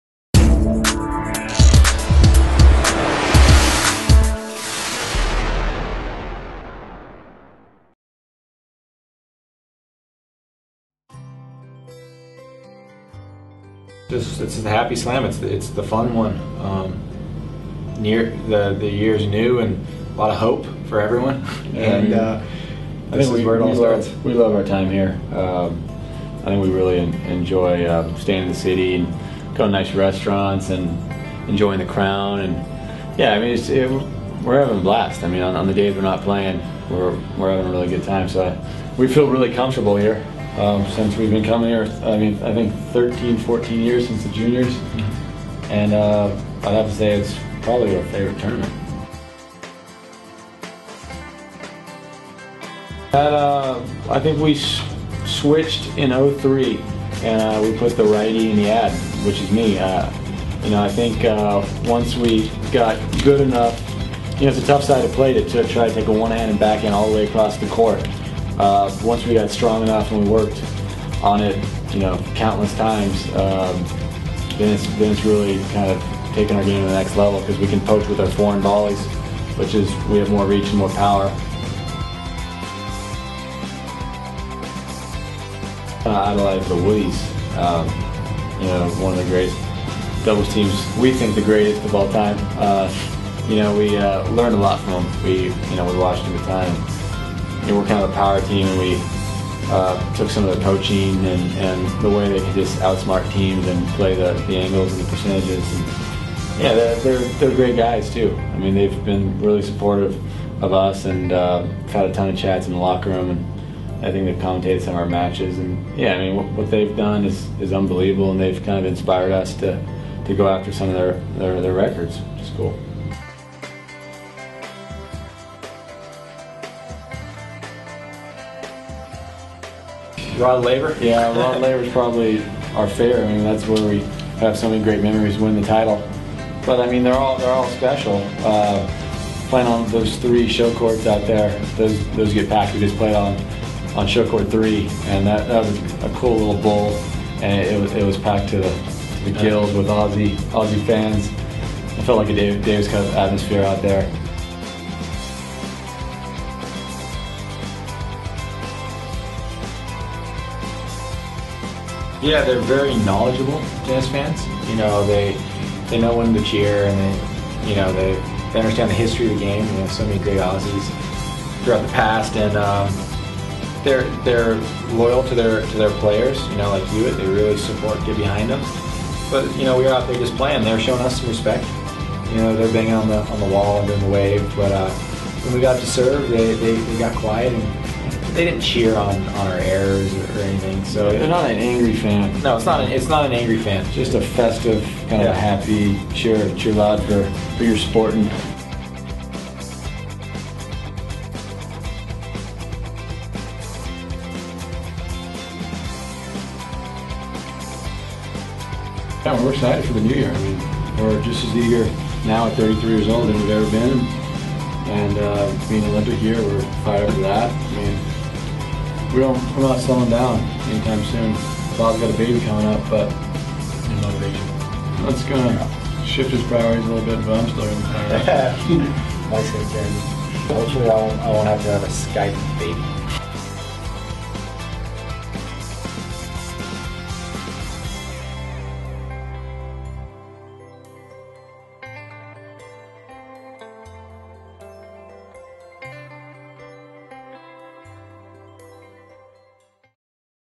AO Insights with the Bob & Mike Bryan
Bob & Mike Bryan take us inside their Australian Open experience